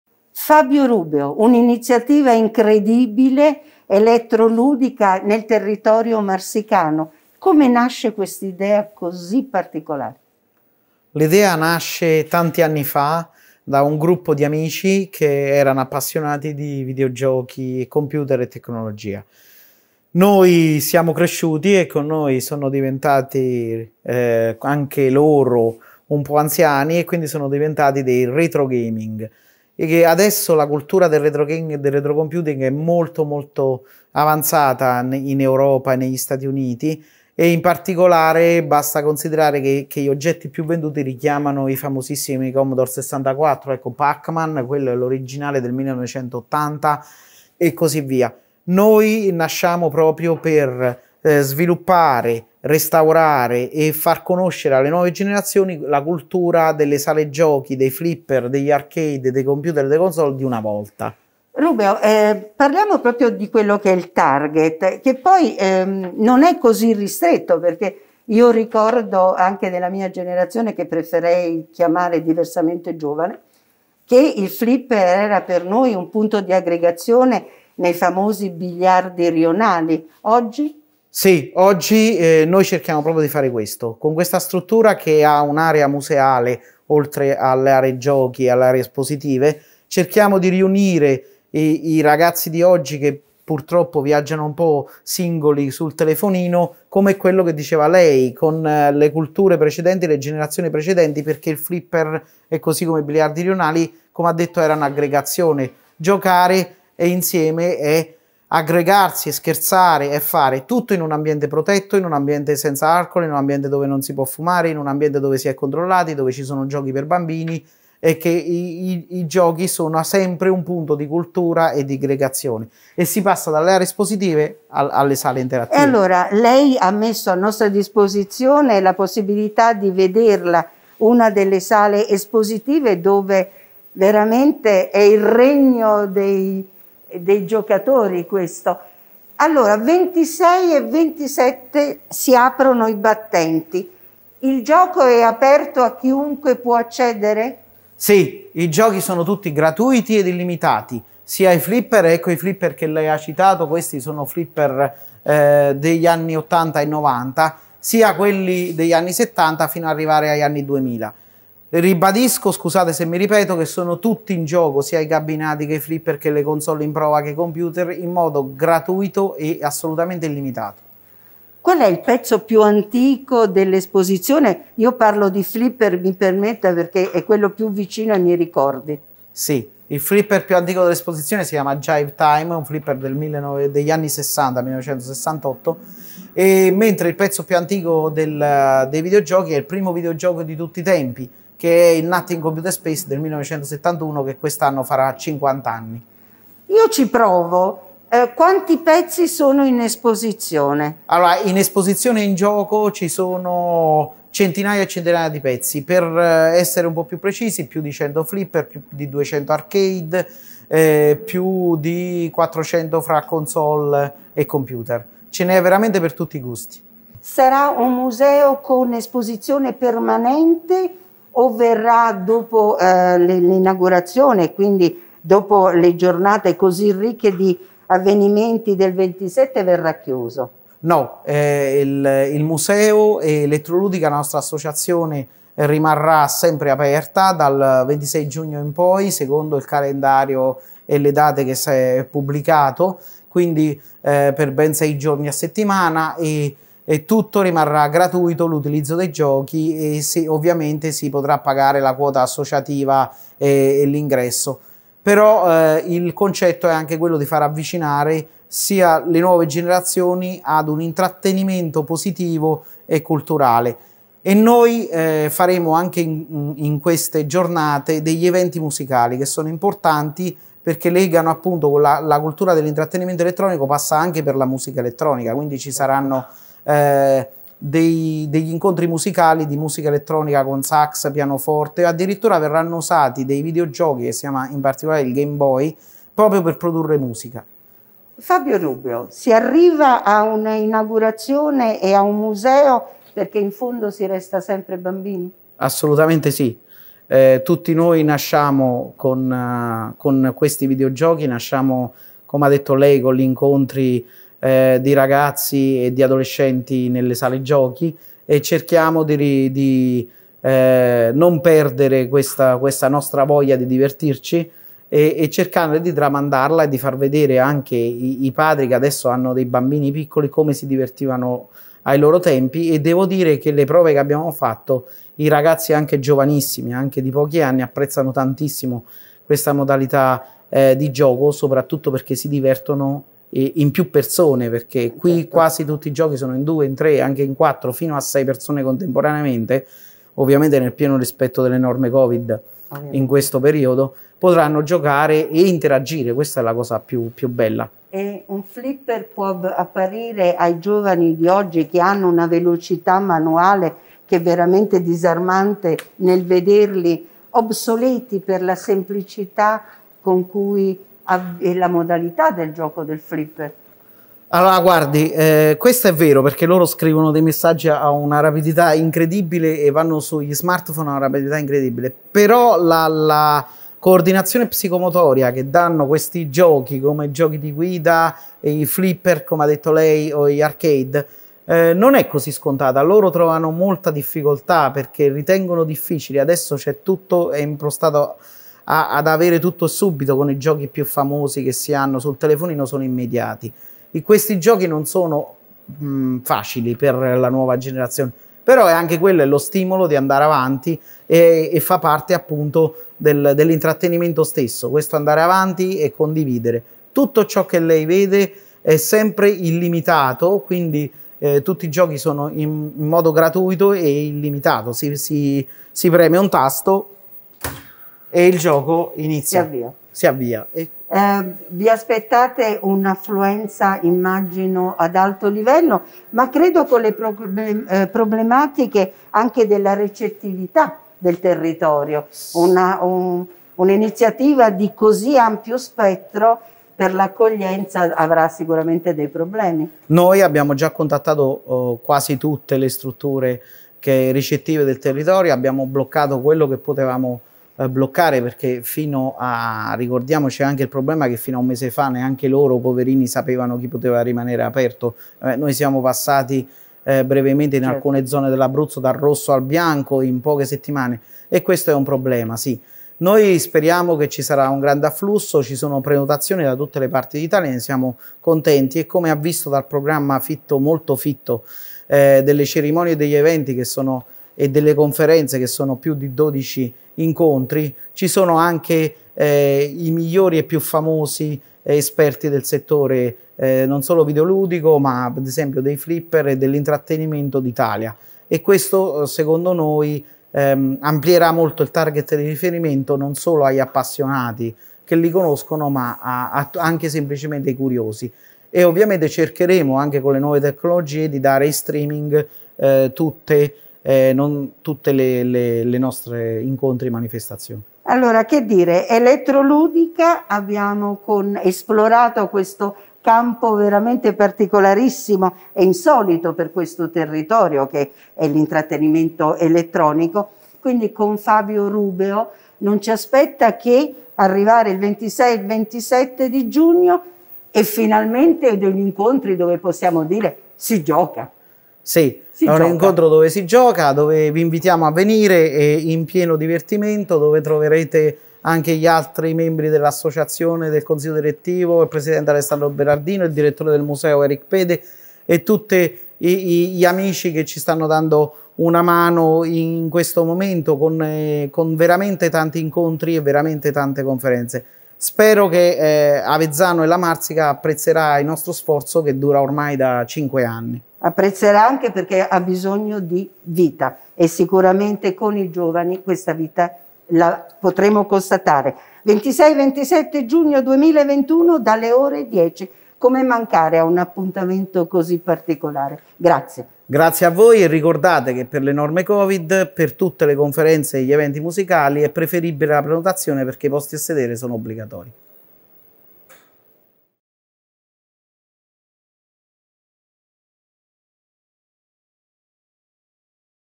elettroLudica ultimi preparativi prima dell'inaugurazione
Avezzano, 10 giugno 2021 - Luisa Novorio intervista Fabio Rubeo all'interno del Museo dell'Intrattenimento Elettronico che verrà inaugurato il 26 giugno. Vengono illustrate le iniziative che caratterizzeranno l'attività dell'associazione culturale che ha dato vita all'esposizione più grande d'Europa di computer, console, cabinati e flipper dal 1971 al 2000.
Video di Marco Di Gennaro